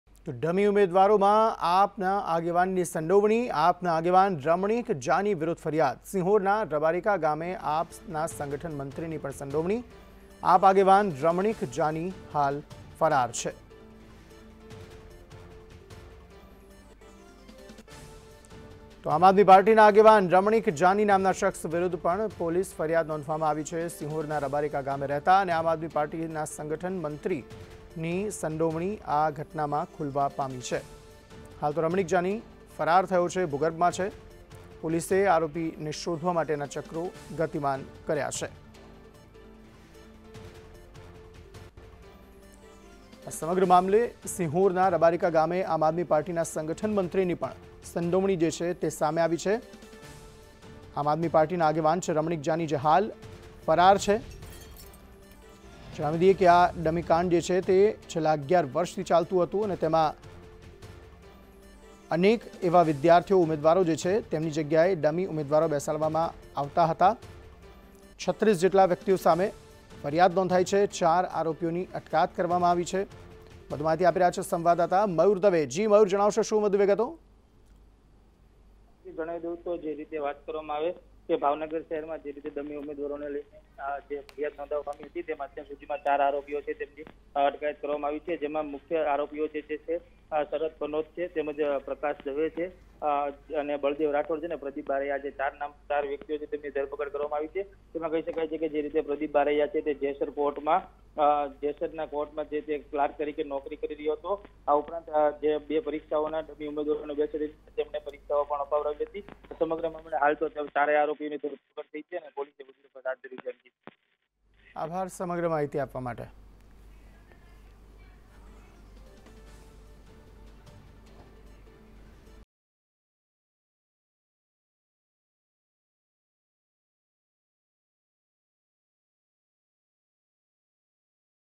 Paper Scam | ડમી ઉમેદવારોમાં AAPના આગેવાનની સંડોવણી
ડમી ઉમેદવારોમાં AAPના આગેવાનની સંડોવણી
AAPના આગેવાન રમણિક જાની વિરૂદ્ધ ફરિયાદ
સિંહોરના રબારીકા ગામે AAPના સંગઠન મંત્રીની પણ સંડોવણી
AAP આગેવાન રમણિક જાની ફરાર